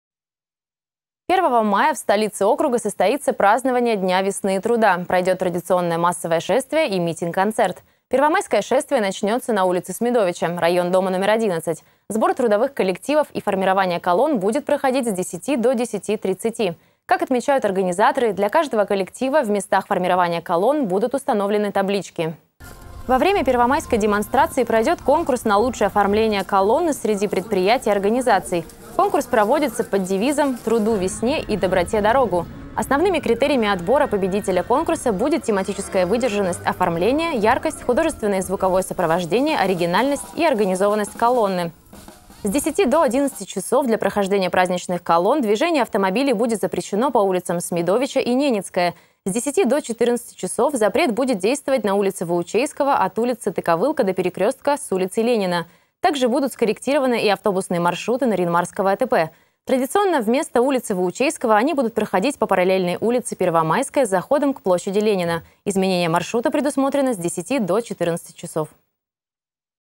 Ограничение движения 1 мая.
1 мая в столице округа состоится празднование Дня Весны и Труда, пройдет традиционное массовое шествие и митинг-концерт. Первомайское шествие начнется на улице Смидовича (район дома № 11). Сбор трудовых коллективов и формирование колонн будет проходить с 10.00 до 10.30. Как отмечают организаторы, для каждого коллектива в местах формирования колонн будут установлены таблички.